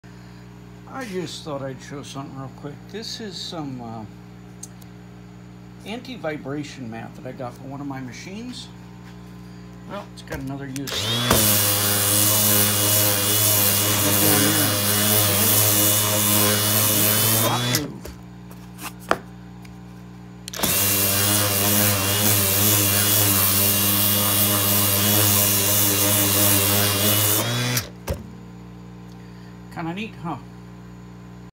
Anti vibration mat sanding surface.
Anti vibration mat makes for great sanding surface.
This is the mat I am using